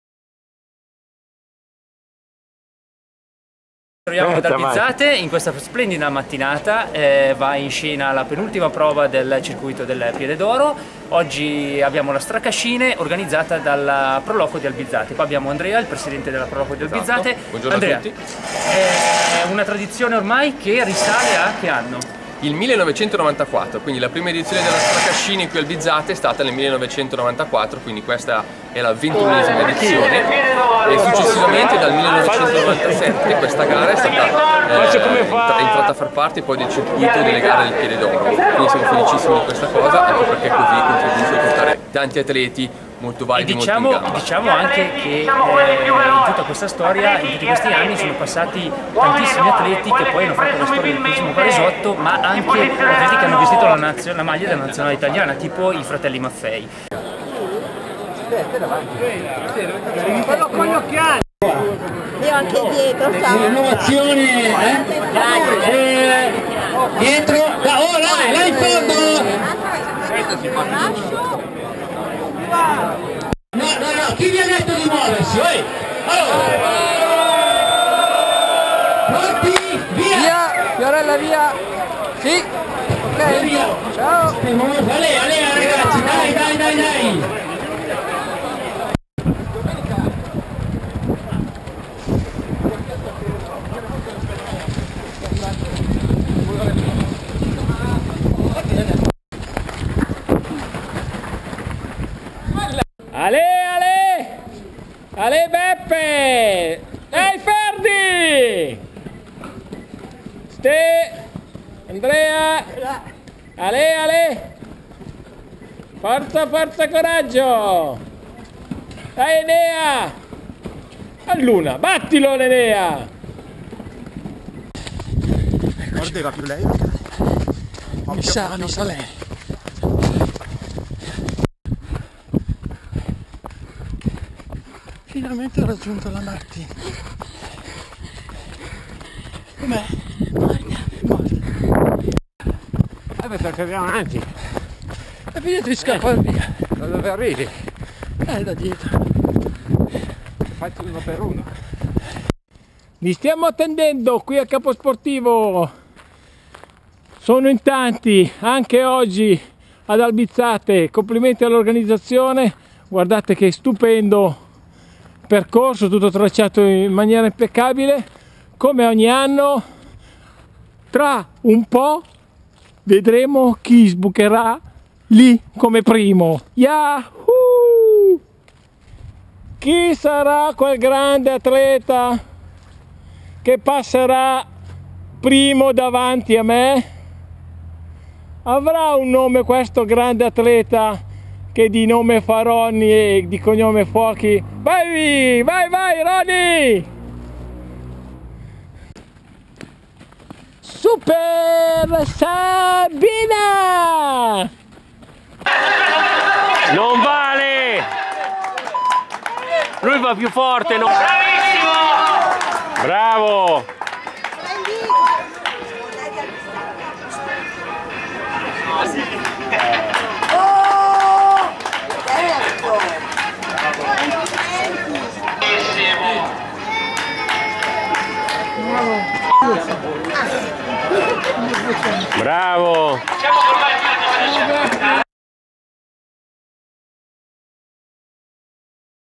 PDO14 ALBIZZATE STRACASCINE
Ventunesima edizione della Stracascine, gara podistica valevole per il Piede d'oro.Giornata di sole e ottima organizzazione della pro loco hanno ospitato gli oltre ottocento runners. Buona visione.